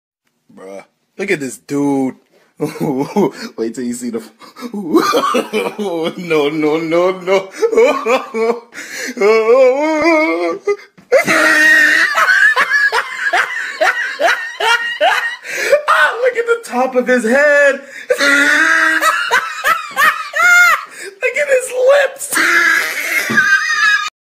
Bruh, look at this dude
I've been a bit too busy to actually animate, so I stayed up until 3 am to make this trash. This is complete shitpost and is for pure entertainment. Hope you enjoy. I own none of the pictures that aren't Hunter. I made the first picture just so I could hecking make this. I'm still proud of it even though it's trash, because this is my trash.